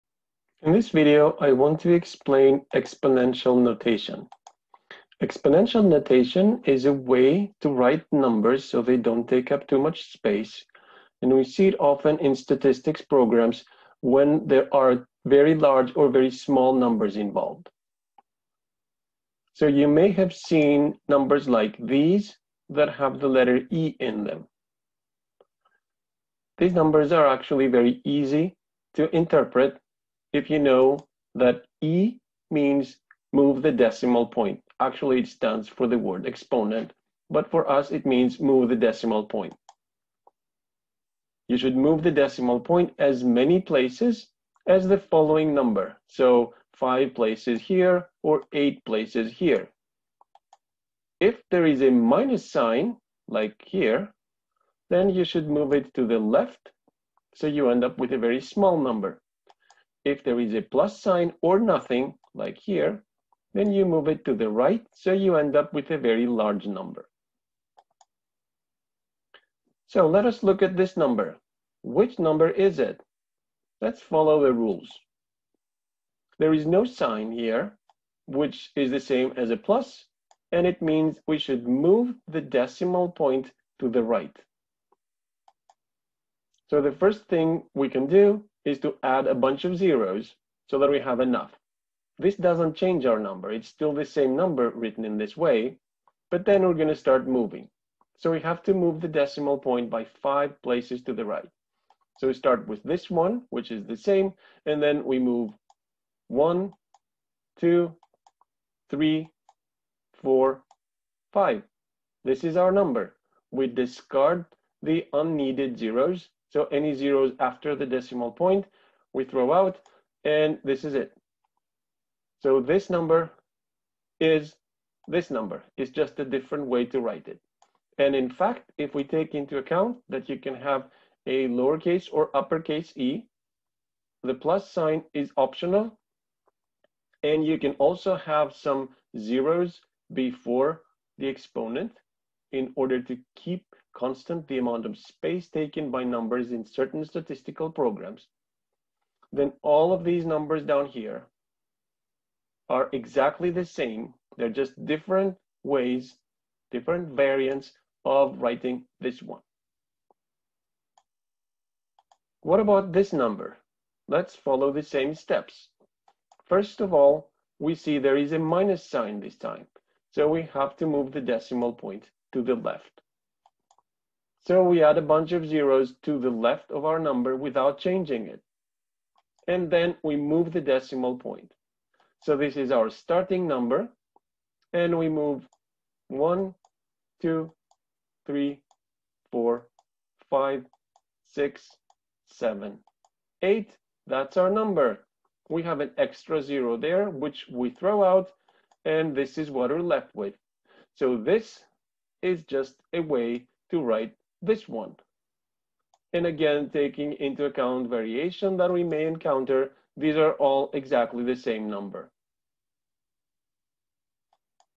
Stats 96 Exponential notation
In this video I explain "exponential notation", that is, the way computer programs (including statistics programs) display numbers that are very large or very small.